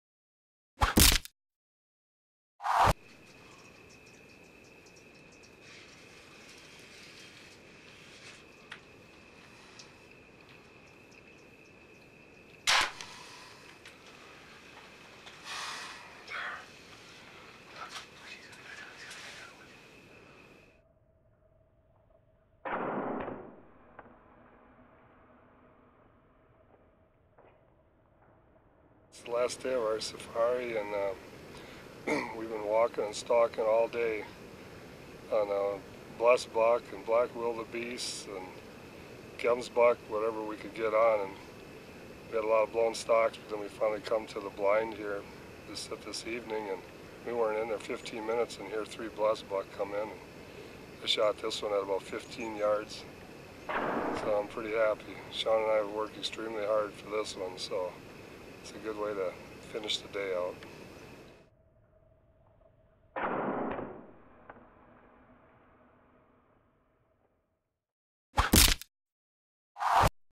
Blesbok Bow hunt on Plains Game Safari in South Africa ARROW IMPACT
Paul Korn, Stewart Korn, and Bob Zaborowski traveled with the gang from A-1 Archery to bow hunt Plains Game in South Africa with Numzaan Safaris out of Thabazimbi.  In this video Bob takes a great Blesbuck at a water hole out of a ground blind with his bow and arrow.  Check out the action along with the arrow impact that was captured in HD.  Congratulations from the gang at A-1 Archery & Tombstone Creek Outfitting